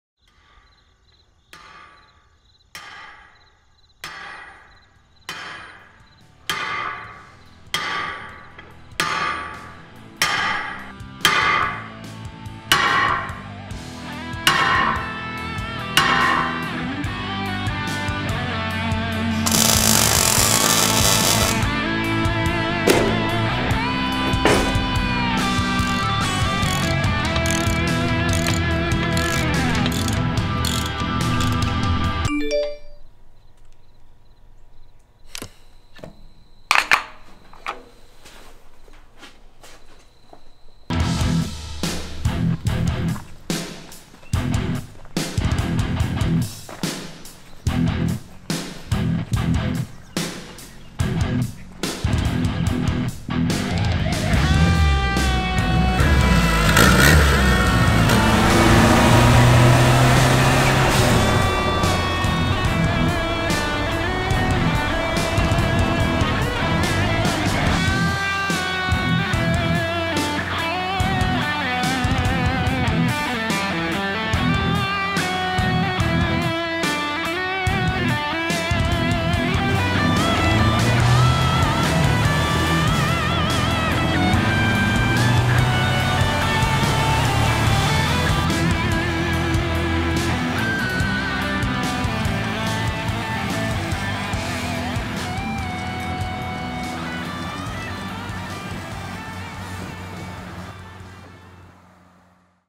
VILLAGE CUSTOMS AND TURBO WILLY RAT ROD TRUCK
Short clip of our Turbo Willy ratrod.